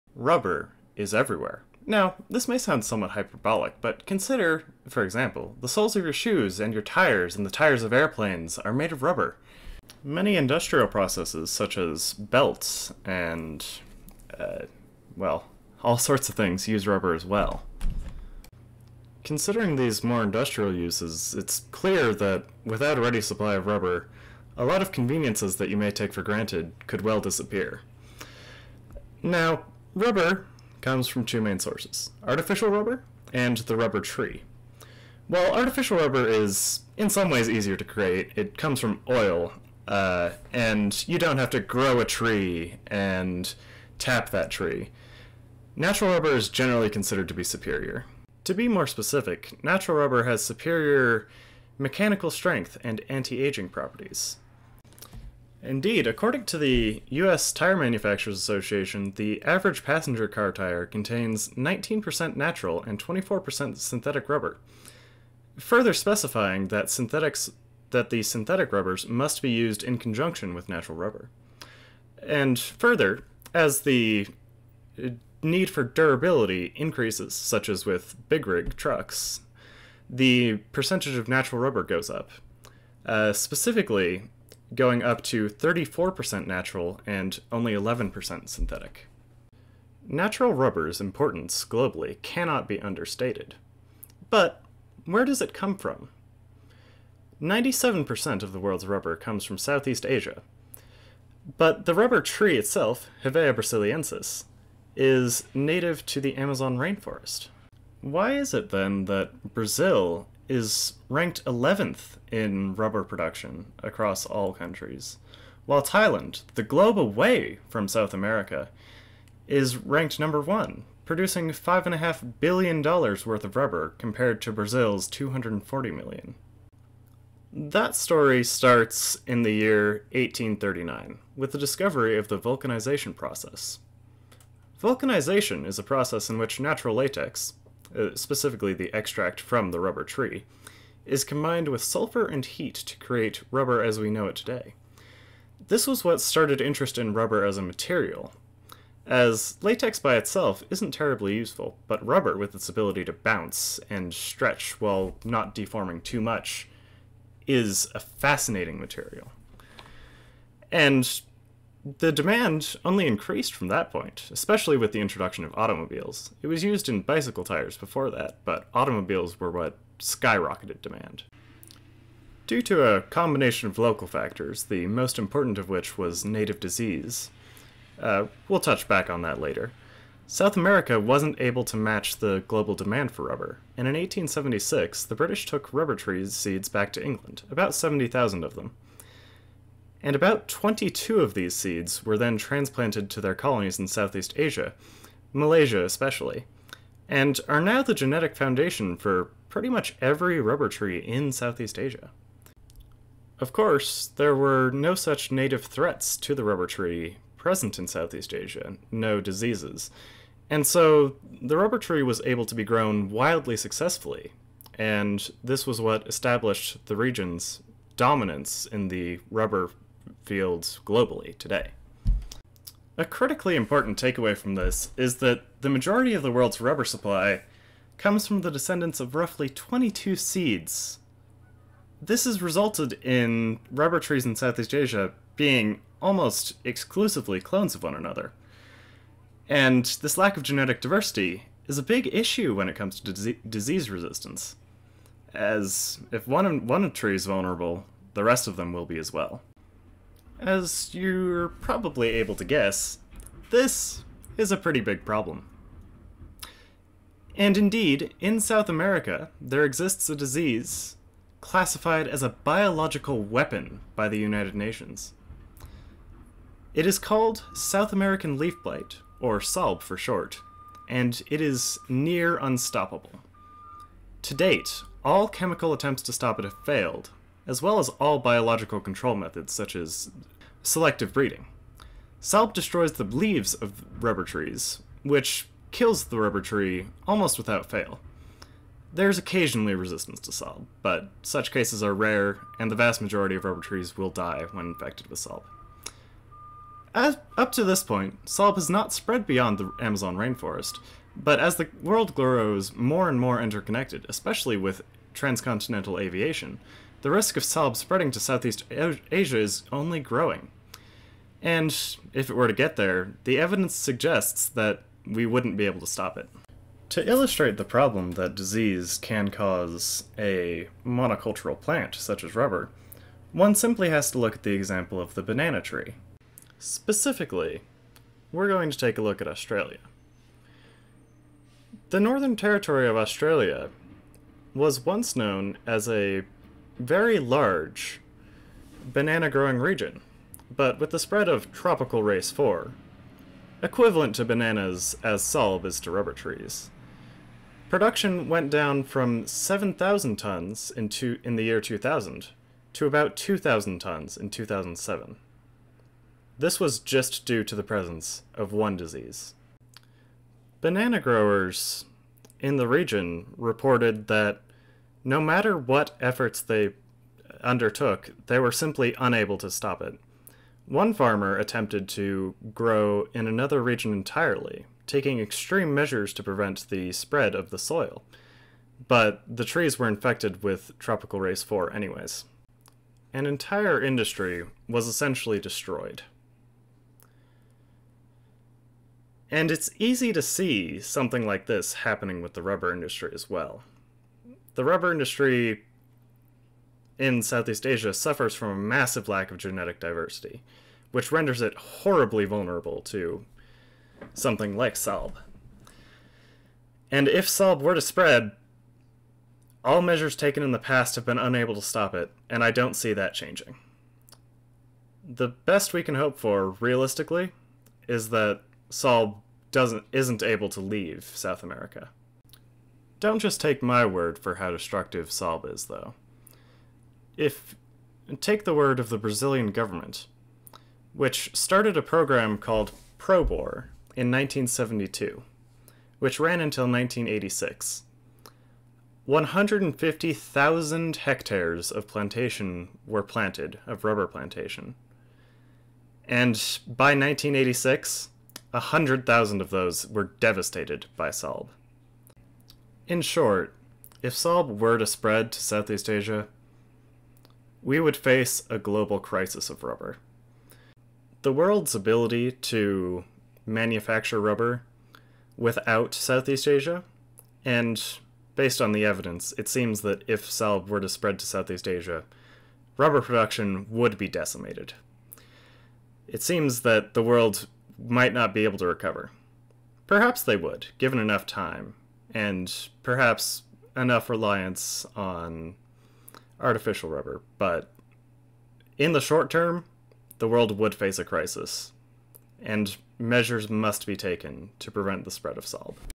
The rubber tree, disease, and the end of the world
This is the result of a semester-long school project!

To be clear: SALB is currently fairly well contained in South America, and measures are already in place to prevent its spread. There is not reason to panic, as of right now.